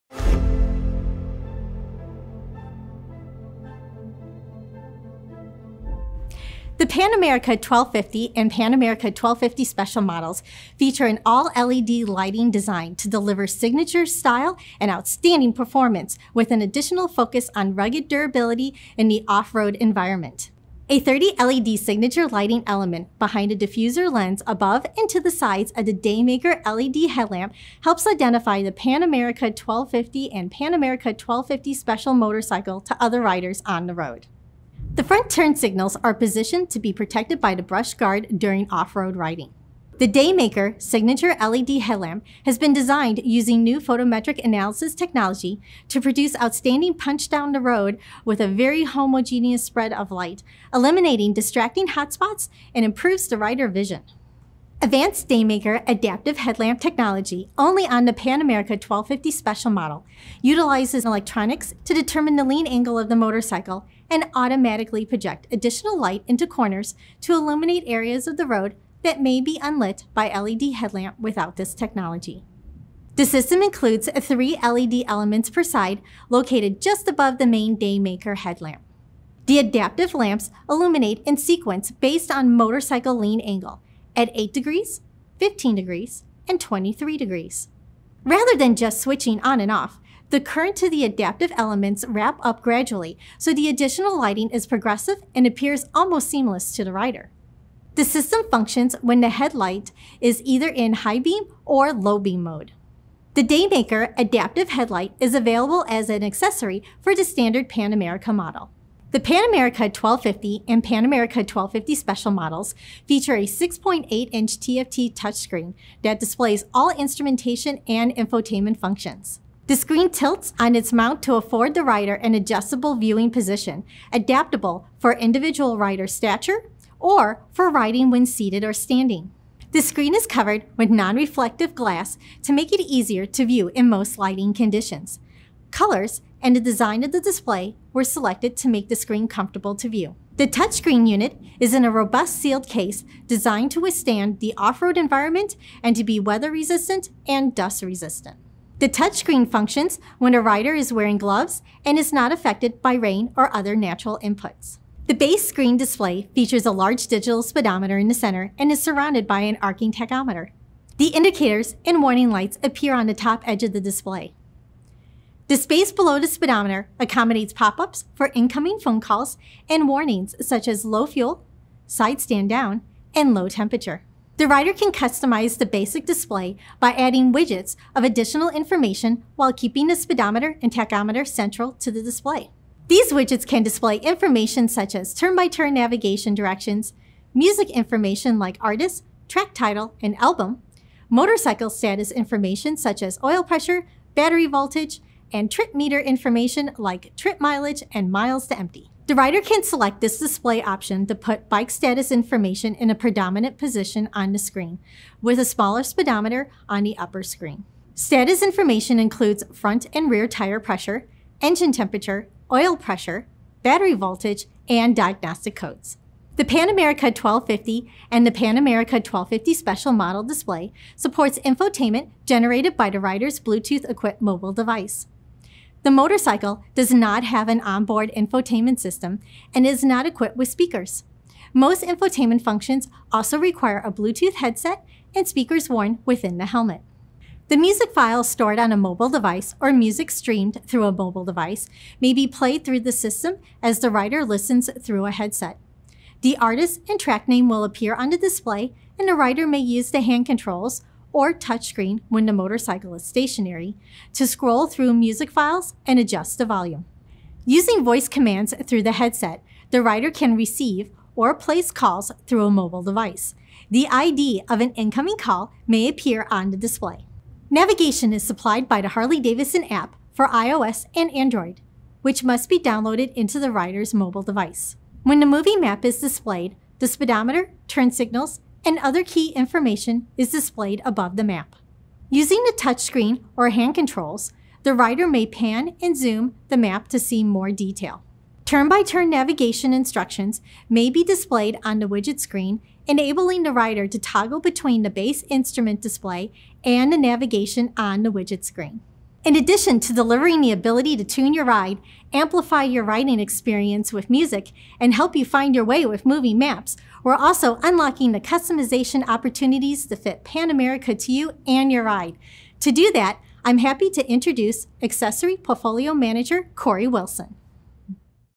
Phần 6: Hệ thống đèn LED và các tiện ích giải trí | Pan America Stories
Với hệ thống đèn LED chiếu sáng thế hệ mới Pan America có thể chinh phục nhiều dạng địa hình khác nhau kể cả khi trời tối. Hệ thống đèn này còn là một trong những nét đặc trưng tạo nên tính nhận diện cao đối với Pan America.

Bên cạnh đó, chiến mã này còn được trang bị hàng loạt tiện ích đỉnh cao, giúp bạn tận hưởng tối đa trên các cung đường.

Vậy Pan America có những tiện ích nào, hệ thống đèn LED được tạo ra và hoạt động ra sao? Hãy cùng lắng nghe chia sẻ của bà Cindy Miller - Human Factors Engineer - Một trong những người đã góp phần làm nên chiến mã Pan America.

📍 Harley-Davidson of Danang: 84 đường 2 tháng 9, P. Bình Hiên, Q. Hải Châu, Tp. Đà Nẵng - Hotline: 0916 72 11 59